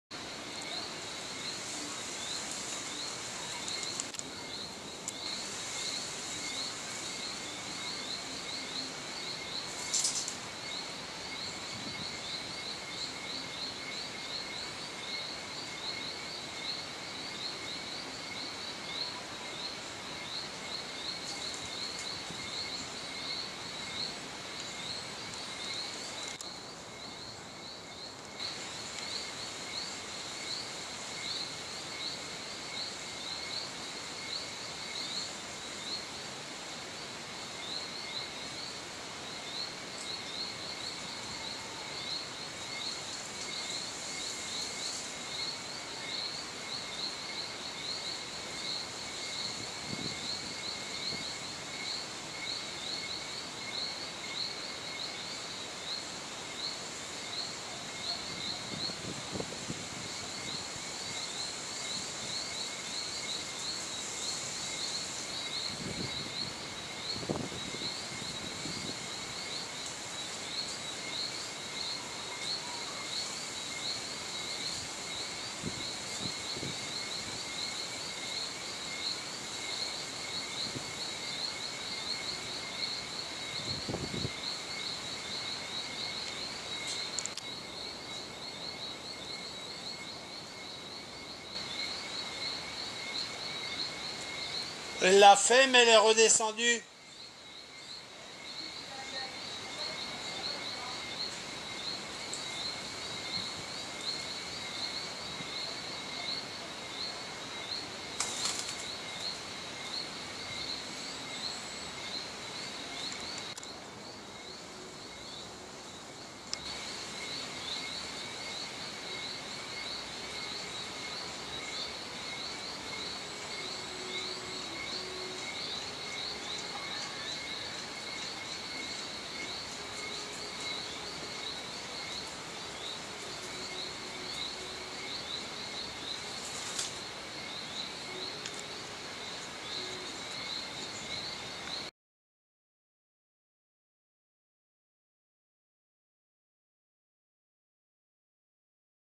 Une poulette de luxe
Ma poulette dort dans un frangipanier et fait le mur tous les soirs